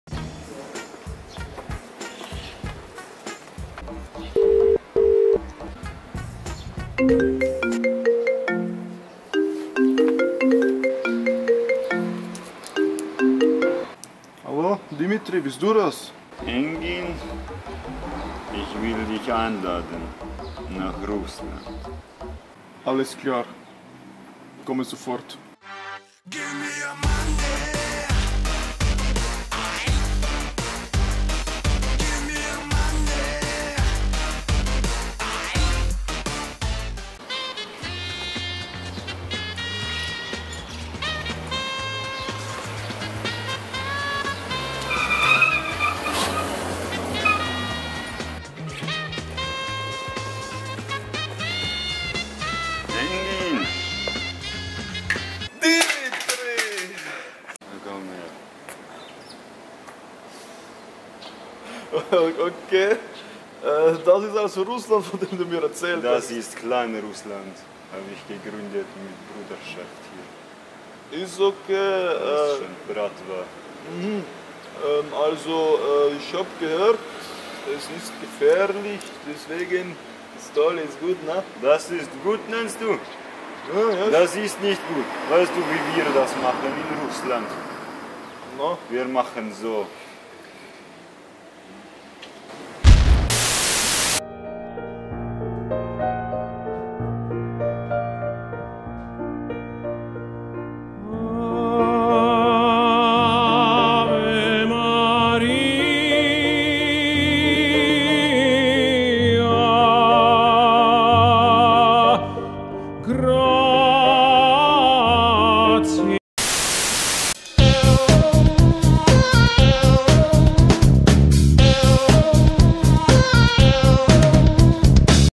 Little Russia (feat. Engin & Dimitri) | HOOD ADVENTURES VOL. 5
Japan Southside hat so einiges an Abenteuern in der Hood zu bieten, weshalb Engin & Dimitri sich gleich auf den Weg machen, um dort so richtig upzuschlackern.